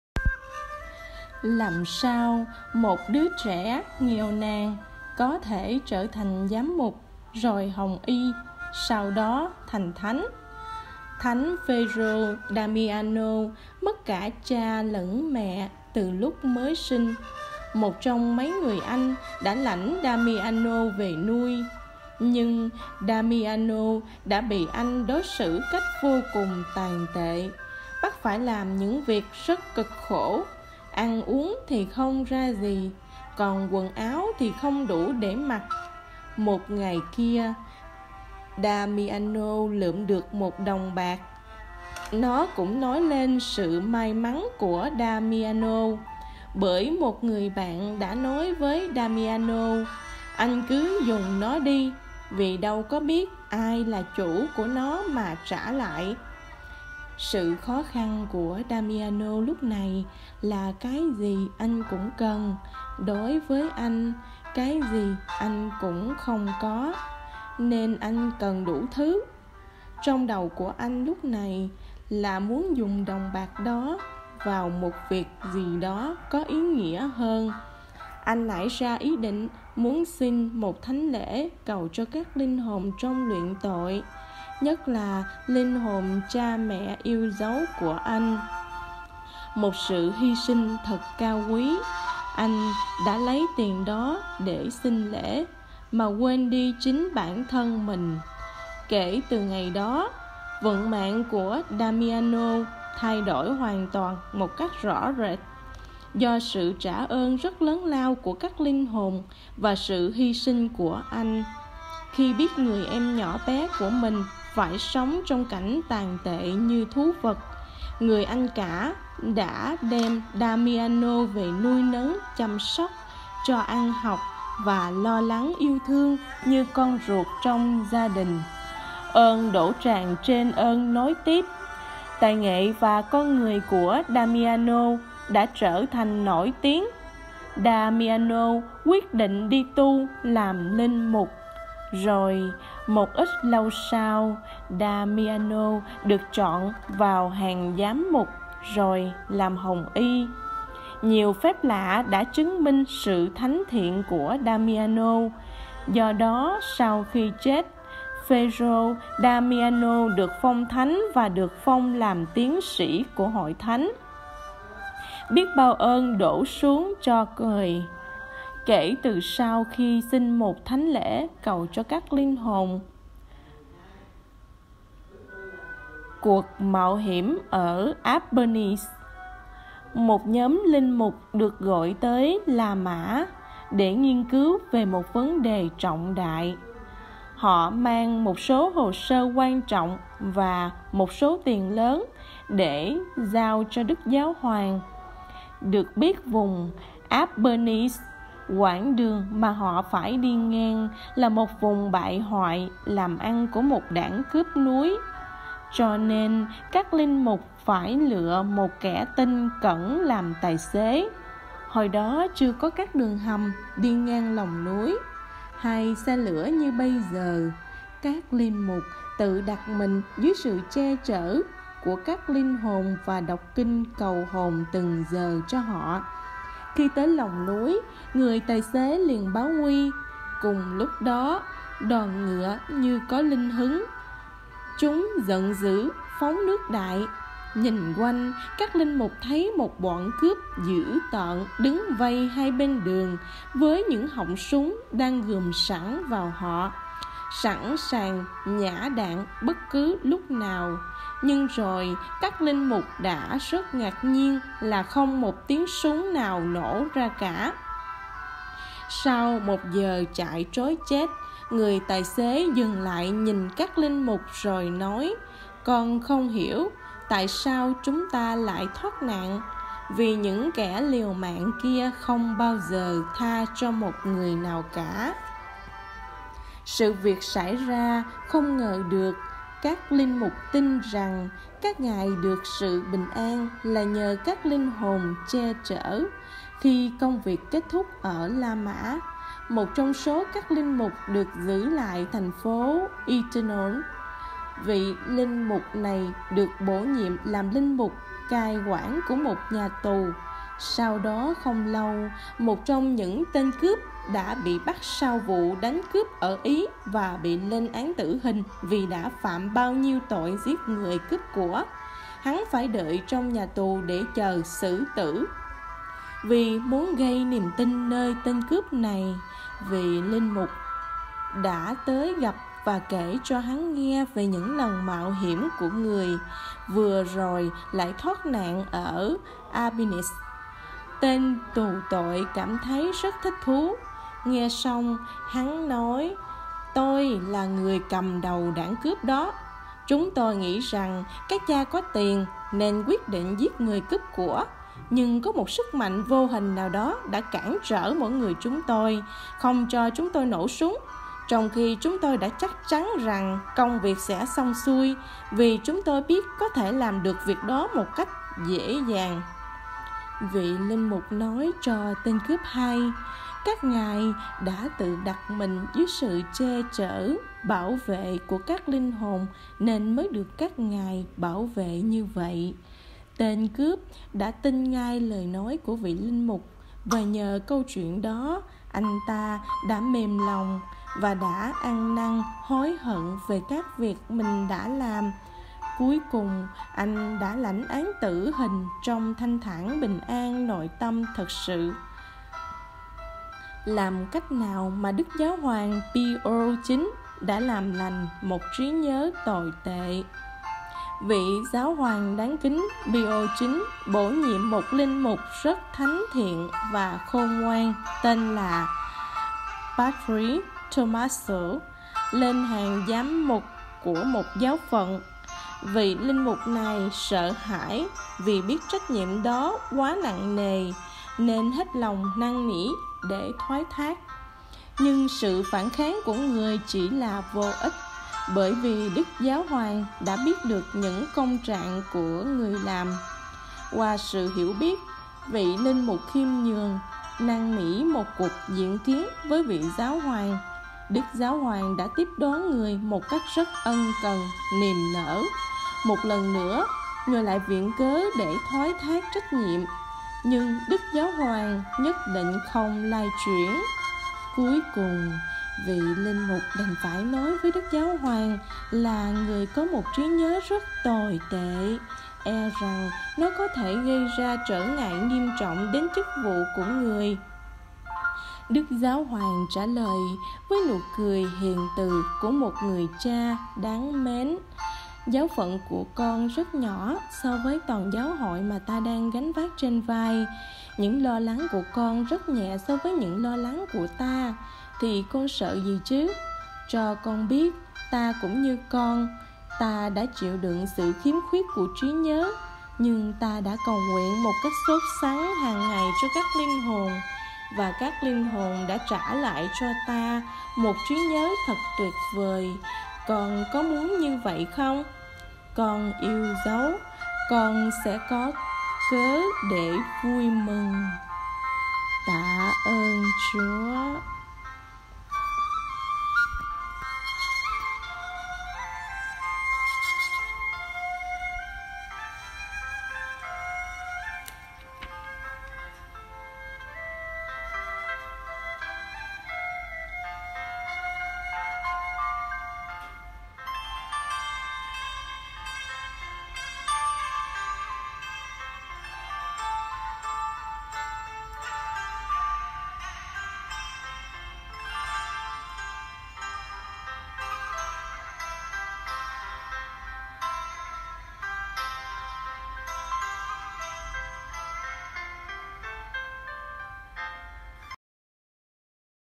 Làm sao một đứa trẻ nghèo có thể trở thành Giám Mục, rồi Hồng Y, sau đó thành Thánh?
Làm sao một đứa trẻ nghèo nàn có thể trở thành Giám Mục, rồi Hồng Y, sau đó thành Thánh?
Cuộc mạo hiểm ở Apennies
Làm cách nào mà Đức Giáo Hoàng Pi-ô IX đã làm lành một trí nhớ tồi tệ?
► (HÃY GIÚP CÁC LINH HỒN- Bản dịch từ sách của Lm. Paul O'Sullivan)

👥 Nguồn ảnh: sưu tầm từ Internet